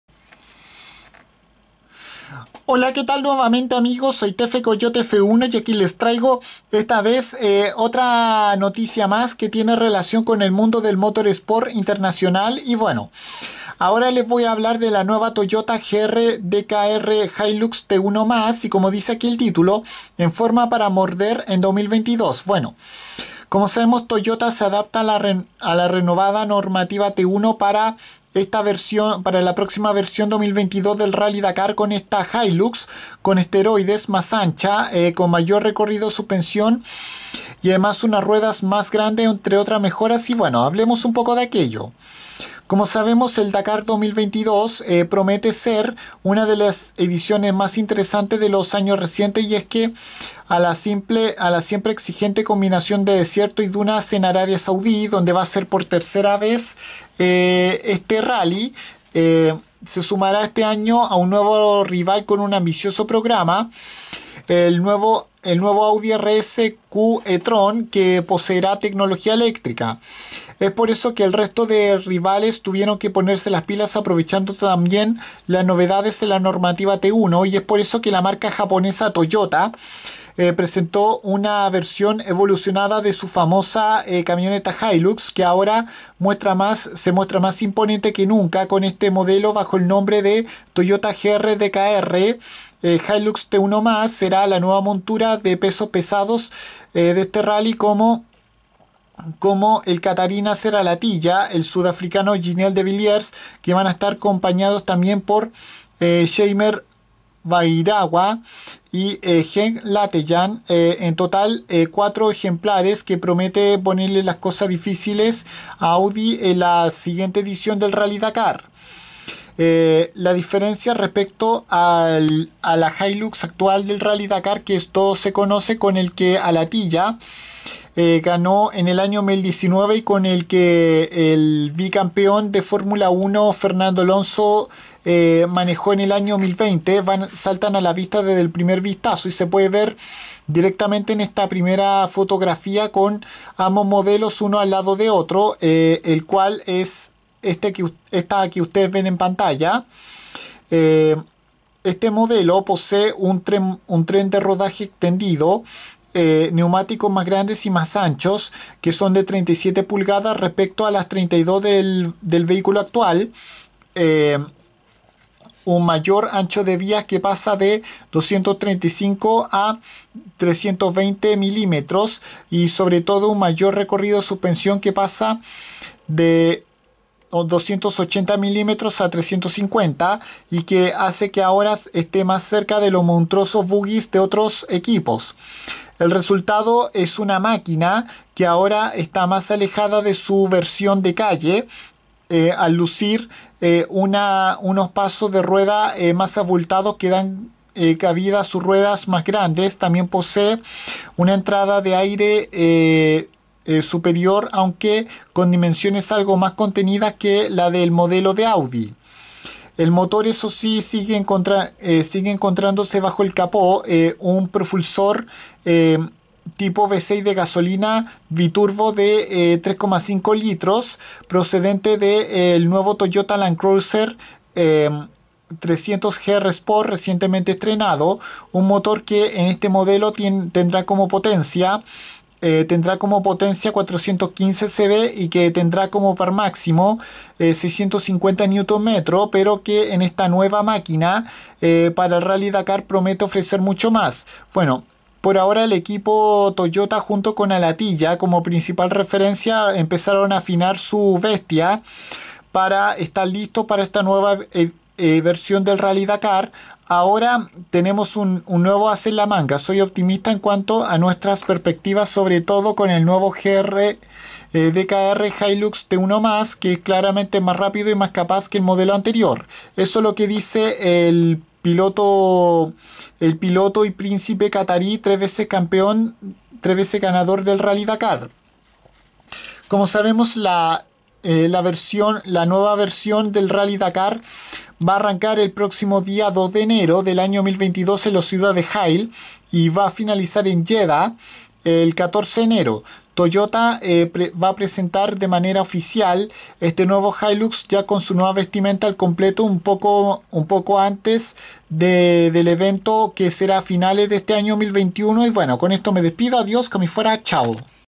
Toyota GR DKR Hilux T1+ información
En este vídeo les comento sobre esta nueva camioneta con la que competirá la automotriz japonesa en la nueva versión 2022 del Rally Dakar que posee muchas mejoras que su antecesor